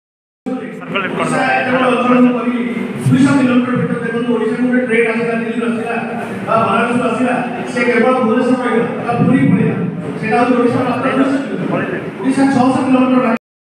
ODF press Conference for establishment of Berhampur Airport
ODF press Conference for establishment of Berhampur Airport at Lions Club, Berhampur